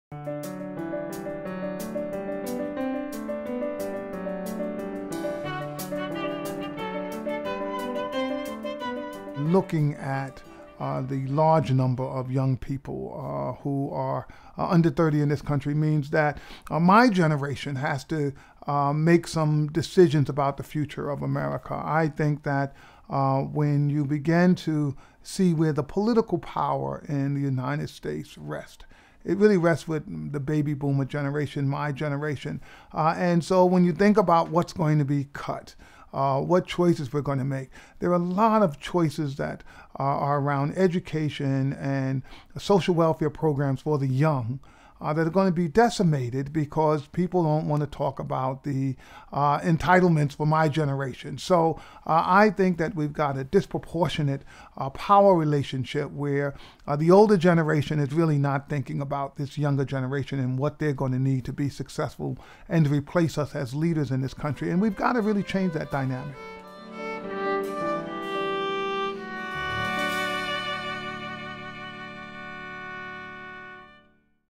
Forgetting the Young Generation - Geoffrey Canada, Harlem Children's Zone USA
Concerned about the decisions taken by the generation currently in power, Geoffrey Canada, President & CEO of the Harlem Children's Zone USA, notices the risk that those currently making the decisions do not take into account the young generation's needs.